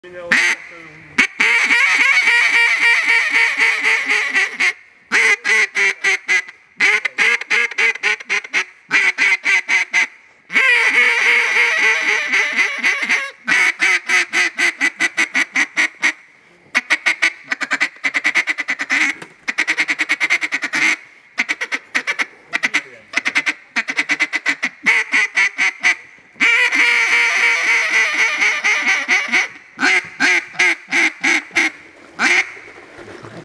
Манок на утку DKC
Многие звуки, которые делает Дима, отлично работали в это утро.
Дима Чернобай после утрянки с манком DKCalls